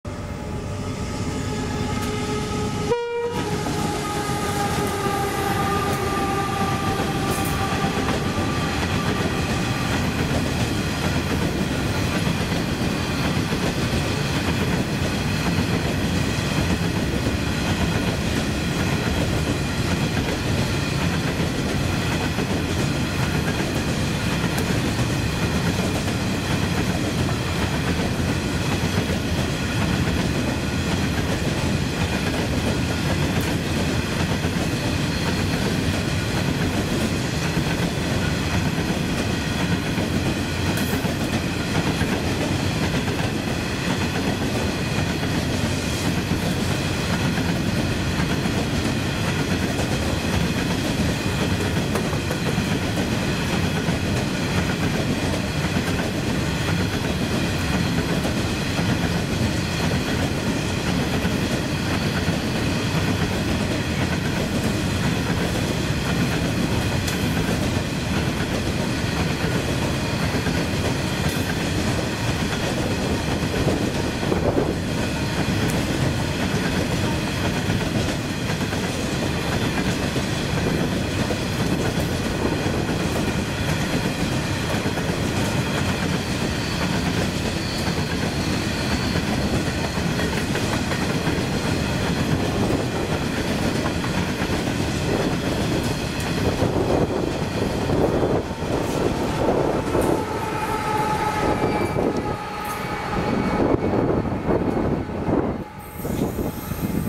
5030 + rear loco 5005 on a down empty coalie cross 5023 + 5042 + rear loco 5002 laoded at Kerrabee
Aurizon New South Wales Coal Trains.